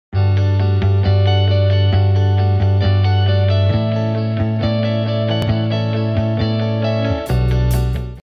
Paisley Raye style video: The Paisley Raye Tulip Tee
Paisley Raye style video. The Paisley Raye Tulip Tee. Watch this short video to see what the Tulip looks like on the plus size body.